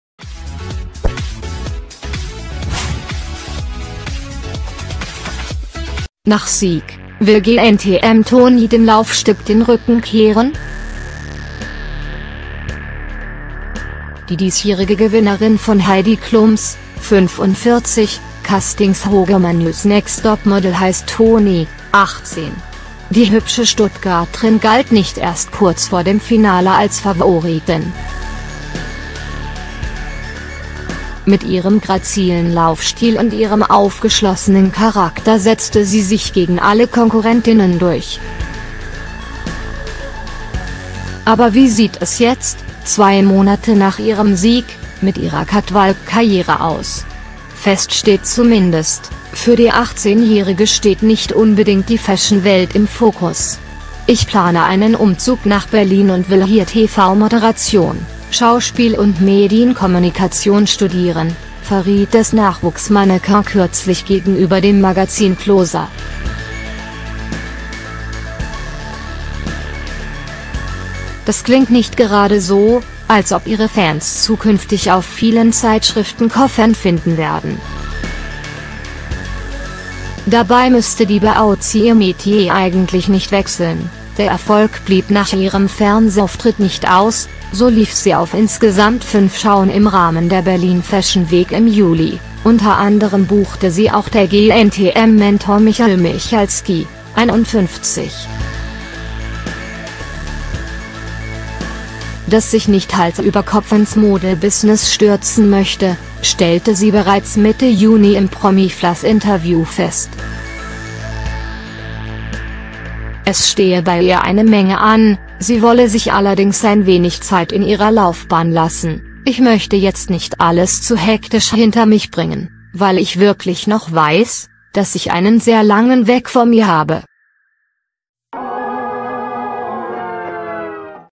Nach Sieg: Will GNTM-Toni dem Laufsteg den Rücken kehren?
Die diesjährige Gewinnerin von Heidi Klums (45) Castingshow Germany's next Topmodel heißt Toni (18)! Die hübsche Stuttgarterin galt nicht erst kurz vor dem Finale als Favoritin. Mit ihrem grazilen Laufstil und ihrem aufgeschlossenen Charakter setzte sie sich gegen alle Konkurrentinnen durch. Aber wie sieht es jetzt, zwei Monate nach ihrem Sieg, mit ihrer Catwalk-Karriere aus? Fest steht zumindest: Für die 18-Jährige steht nicht unbedingt die Fashion-Welt im Fokus!